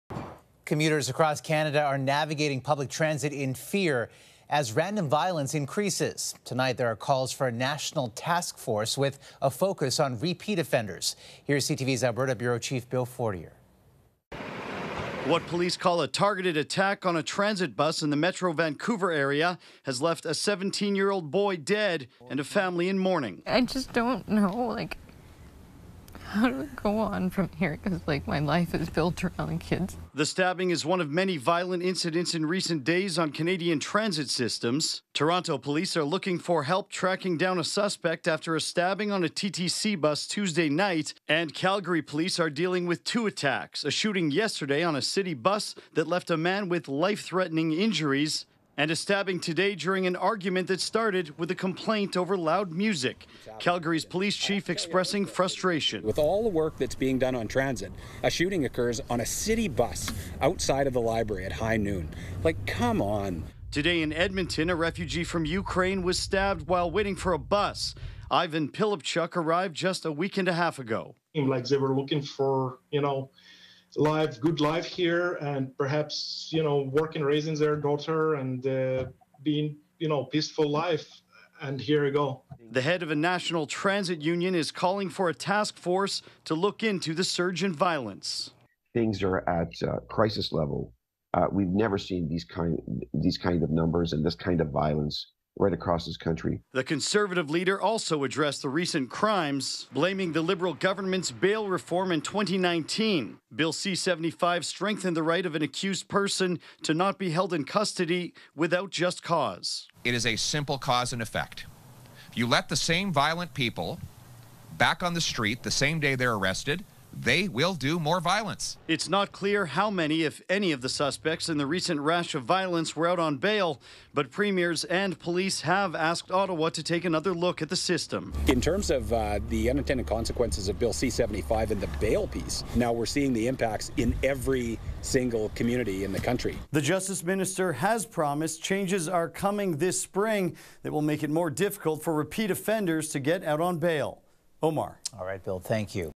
Transit attacks | Violent incidents Increasing across Canada
Bill Fortier reports on the recent string of attacks on transit systems across Canada including in Toronto, British Columbia and Alberta.